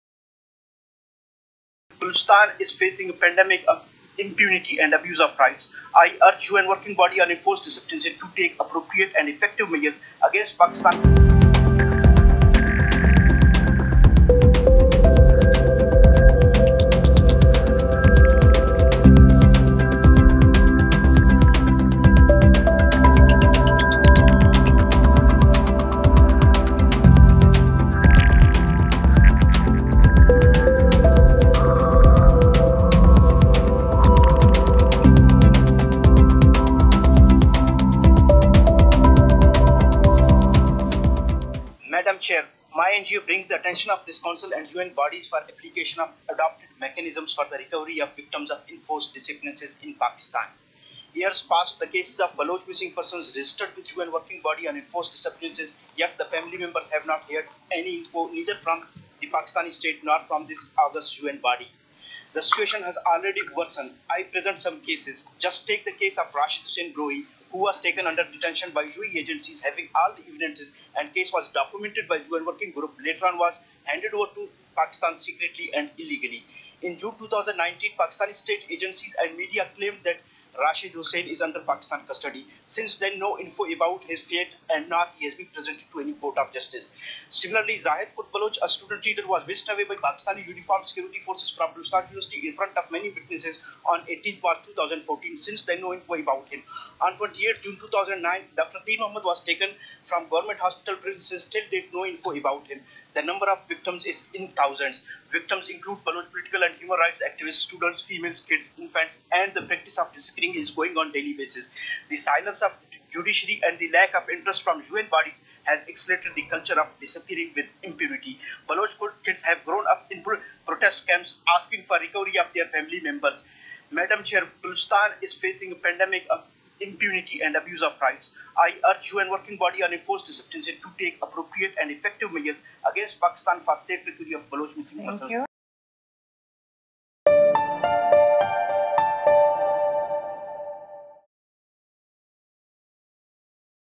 ‘Balochistan facing pandemic of impunity’: Activist exposes Pakistan at UNHRC
Activist Munir Mengal has exposed Pakistan’s atrocities in Balochistan during his speech at the UNHRC. Her said that enforced disappearances continue unabated and with absolute impunity. He added that Pakistan or UN body has not helped people of Balochistan and urged people to take notice of what has been happening in the region. Mengal further said that the number of victims is in thousands and include Baloch political and human rights activists, students, females, kids and even infant and called for action from UN bodies.